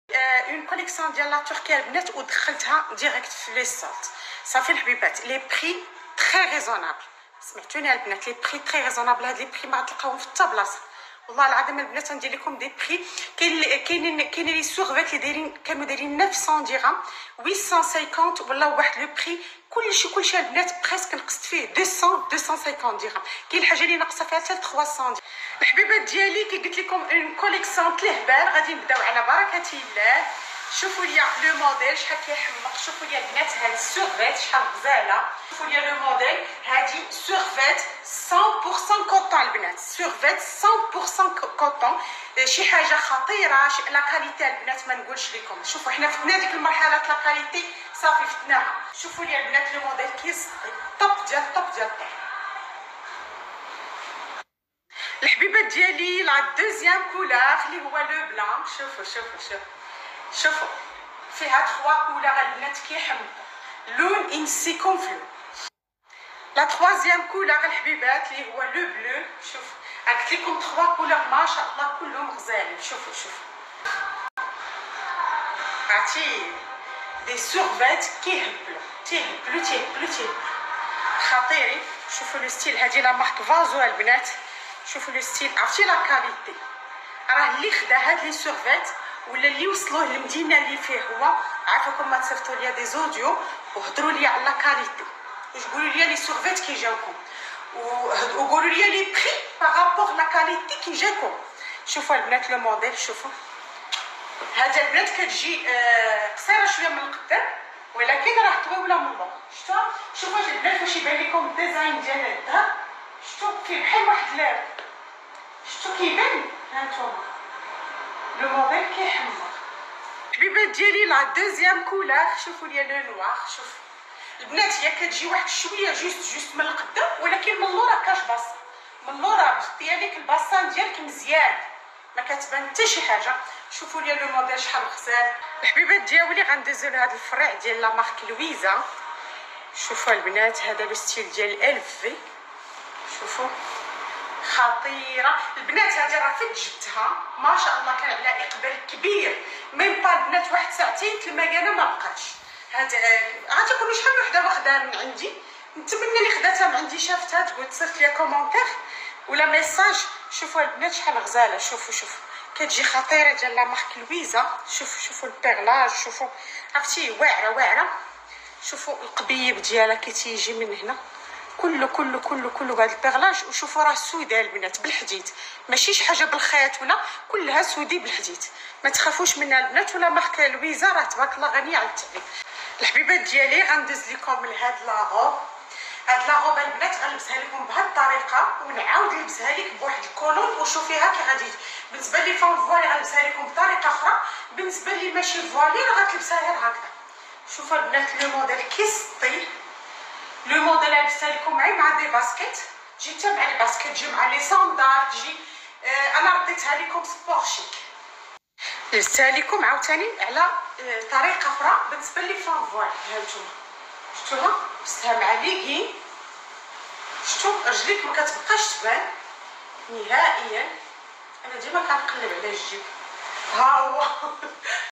وأخيرا سيدة شوبينغ كترجع ليكم بالجديد ملابس تركية لفصل الصيف راقية وبأثمة جدا مناسبة
جديد الملابس التركية لفصل الصيف موضة 2024 عند سيدة شوبينغ أمال